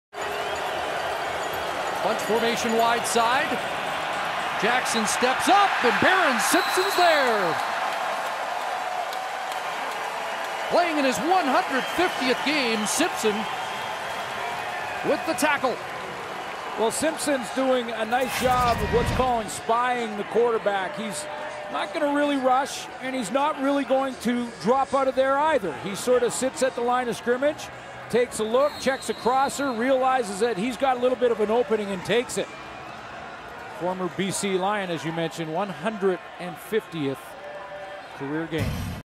Barrin Simpson sacks Jarious Jackson - August 12, 2010
August 12, 2010 - Barrin Simpson sacks Jarious Jackson in Regina.